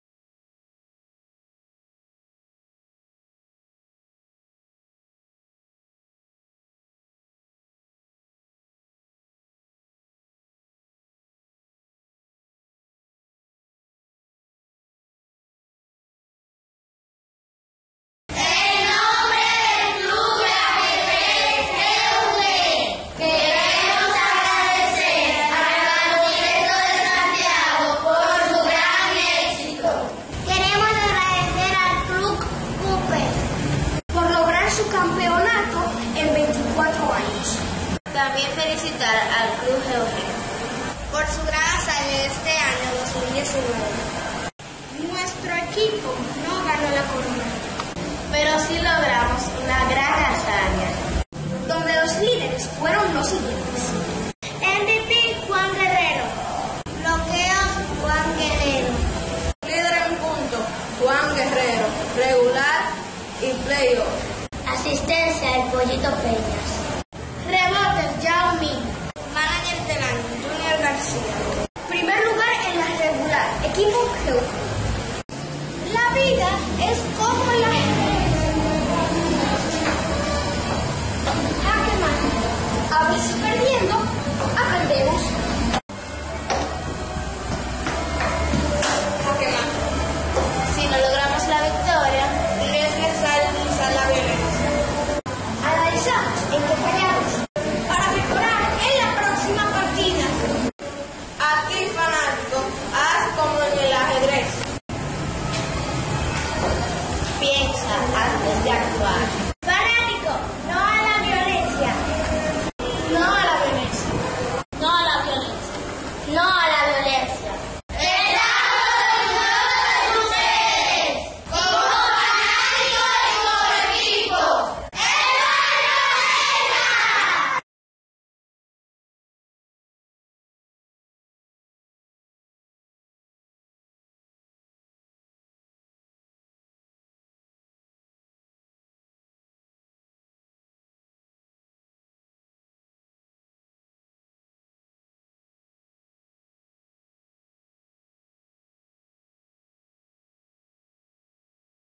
agradecimiento y mensaje al baloncesto de santiago
de parte de los niños de ajedrez de la fundación FE del club G.U.G